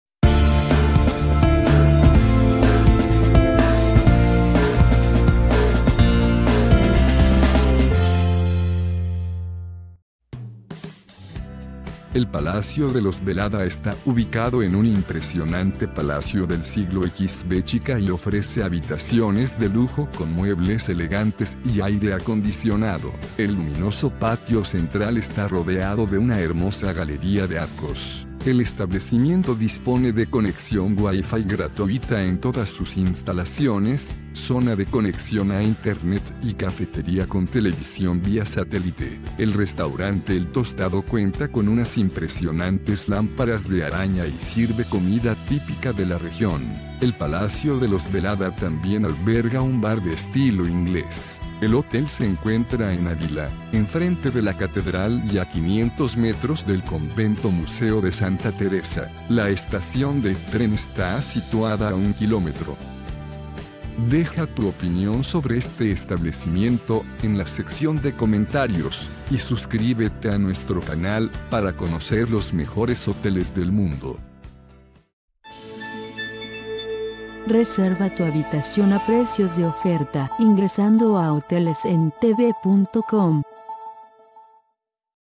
Hotel Palacio de los Velada en Avila
Palacio de los Velada de categoría 4 estrellas cuenta con restaurante, servicio de habitaciones y salas de reuniones / banquetes.